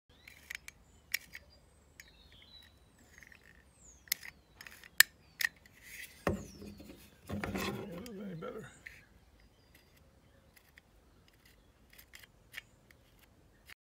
Egg cutter
Some egg cutters are a ring with teeth on the inside circle. You press on two handles on either side of the outside of the ring, which presses the teeth in on the egg to cut it.